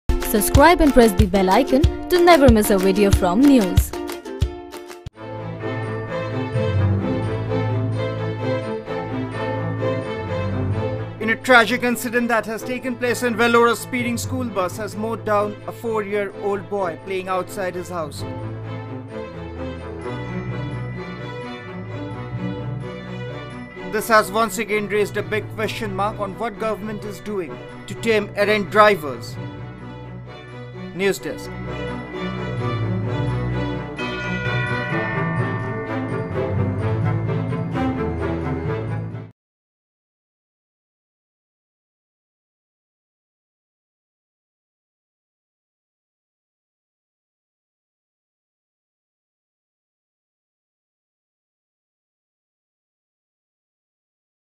Caught On Cam: School Van Mows Down 4-Year-Old in Vellore | NYOOOZ TV
In a tragic incident that has taken place in Vellore, a speeding school van has mowed down a four-year-old boy playing outside his house. This once again raises a big question mark on what government is doing to tame errant drivers teaching them to slow down in the residential areas.